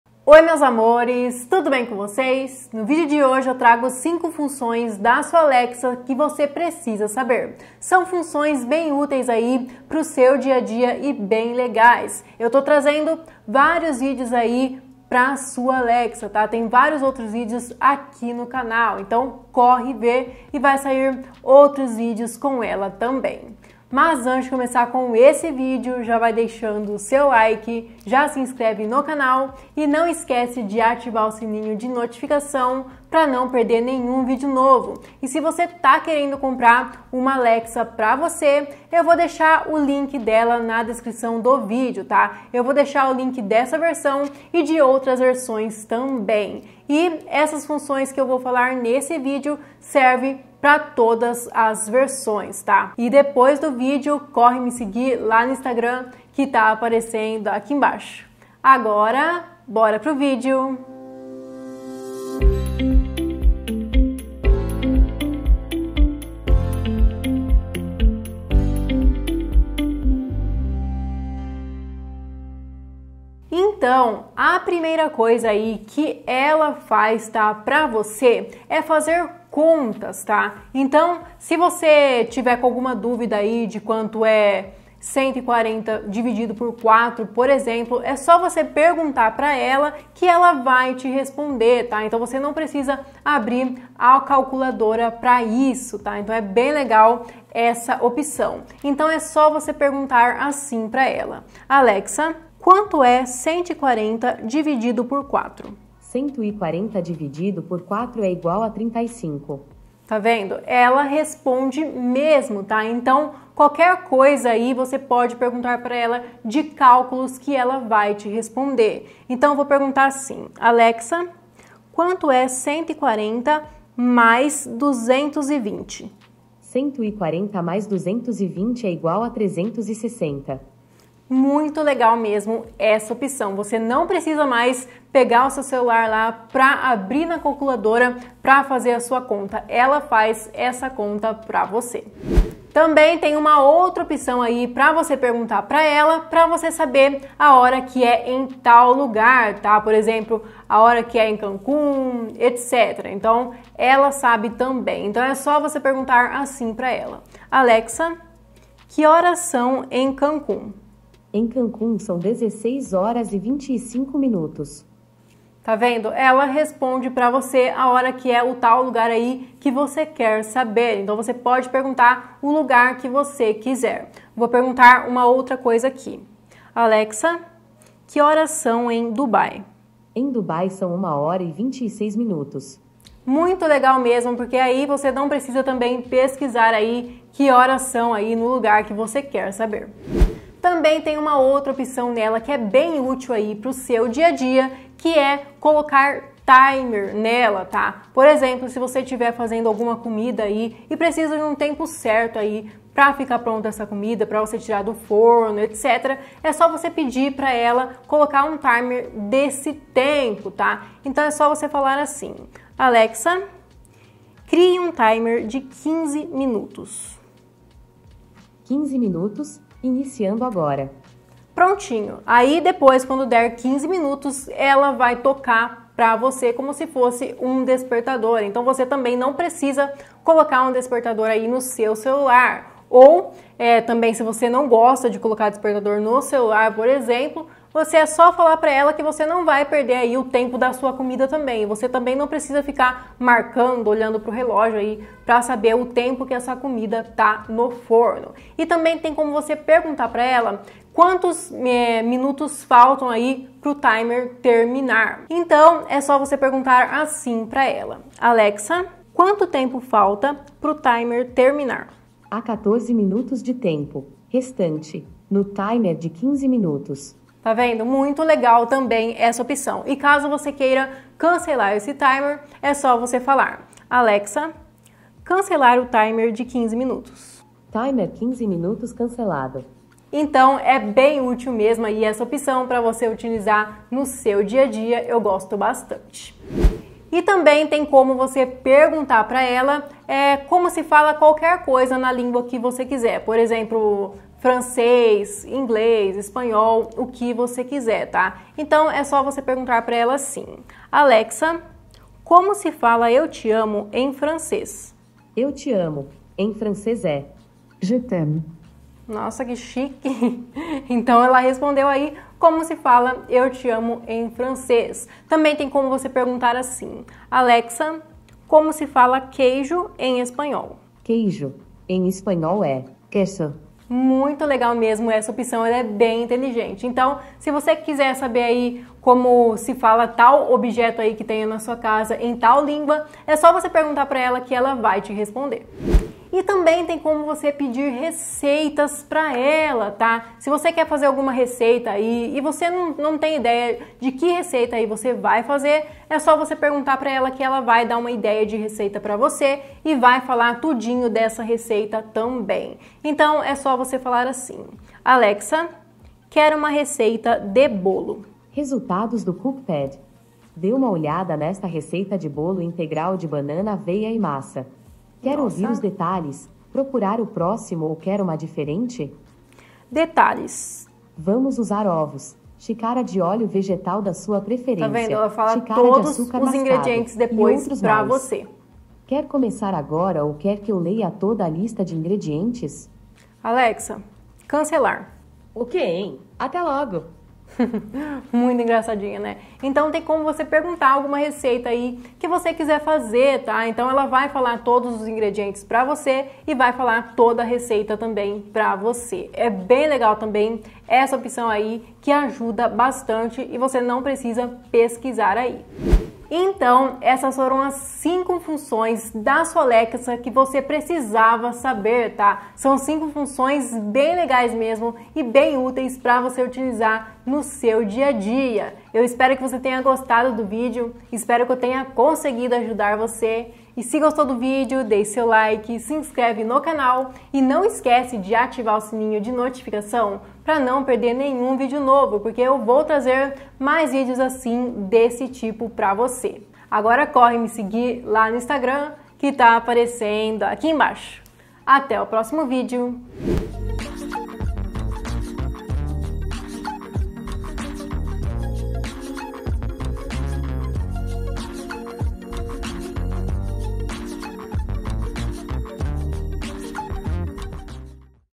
TOP 5 FUNÇÕES da SUA ALEXA que VOCÊ PRECISA SABER!
📌Echo Dot Alexa!

Oi meus amores, nesse vídeo eu trago 5 Funções pra sua Alexa que você precisa saber!

📌COMPRE SEU SMARTPHONE NO BRASIL SEM TAXAS COM O MELHOR PREÇO!

🔥Ofertas diárias na Amazon Brasil! APROVEITE

📱 SIGA-ME!